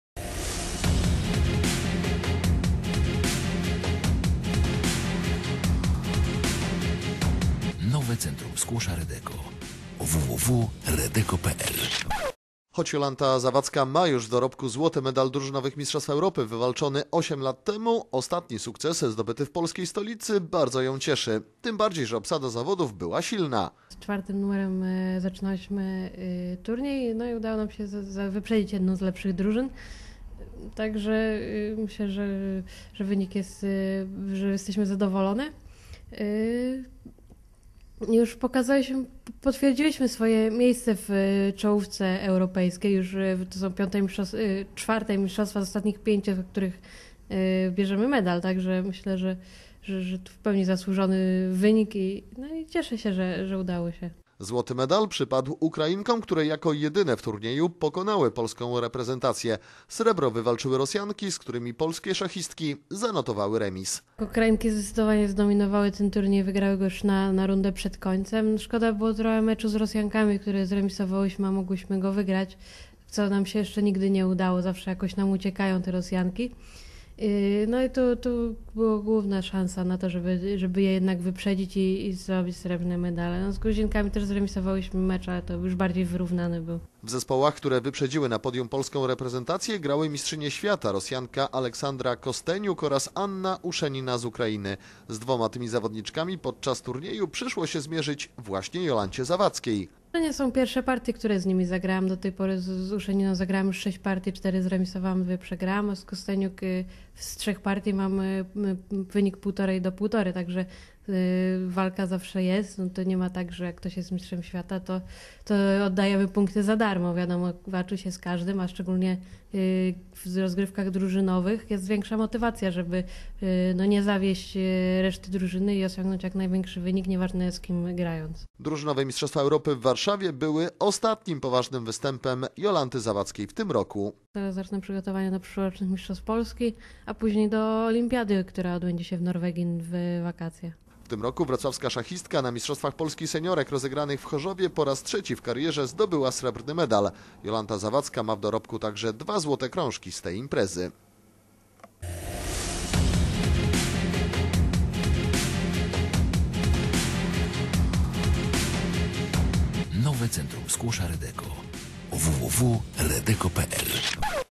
Jolanta Zawadzka z brązowym medalem ME
Udanie rok kończy czołowa wrocławska szachistka. Jolanta Zawadzka zdobyła czwarty w karierze medal drużynowych mistrzostw Europy. W Warszawie polska reprezentacja, w której grała także inna zawodniczka wrocławskiej Polonii - Iweta Rajlich stanęła na najniższym stopniu podium.